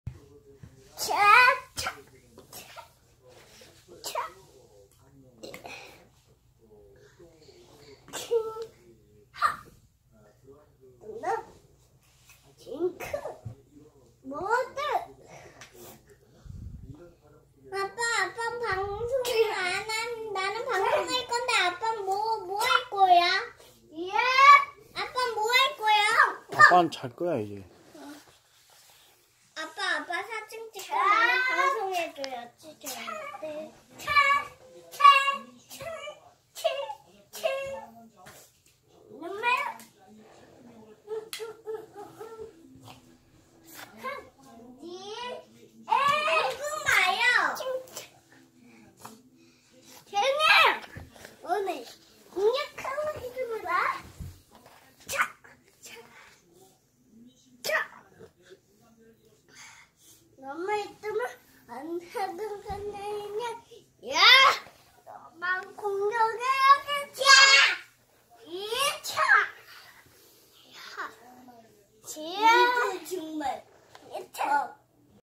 일상에 녹아있는 액션들
우리아들 자기가 로보트인지 무림고수라고 생각하는건지 알수가 없네요